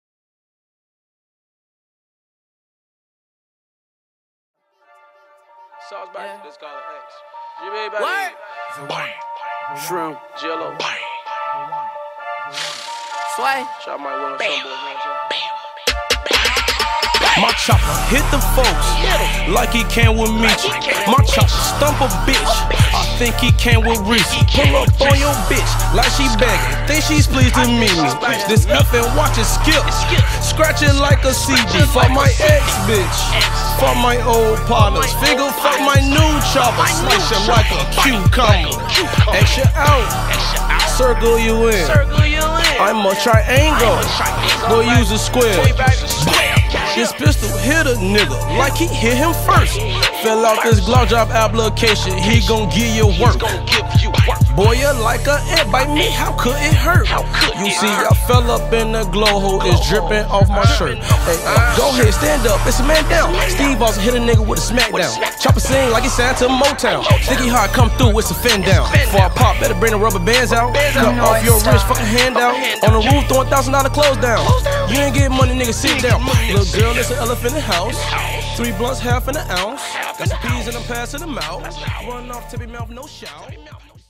GLO X HUF in store signing on Fairfax dir @whoisnorthstar visual prod.
GLO x HUFF cyber Monday relase in store with GBE x GLOGANG visual @ HUF in Hollywood CA